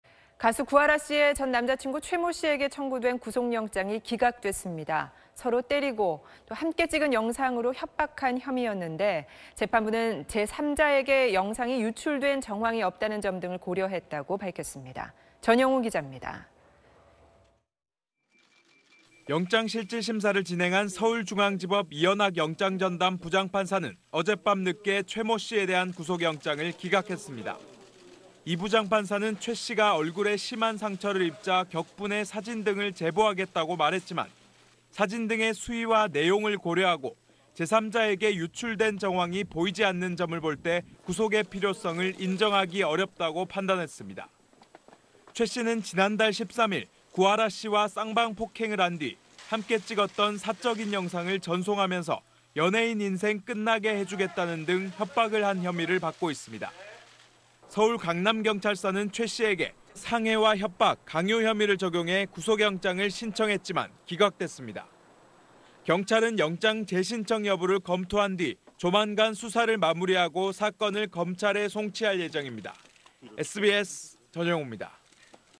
'구하라 협박 혐의' 영장 기각…"영상 유출 정황 없다" / SBS
가수 구하라 씨의 전 남자친구 최 모 씨에게 청구된 구속영장이 기각됐습니다. 서로 때리고 함께 찍은 영상으로 협박한 혐의였는데 재판부는 제3자에게 영상이 유출된 정황이 없다는 점 등을 고려했다고 밝혔습니다.
전형우 기자입니다.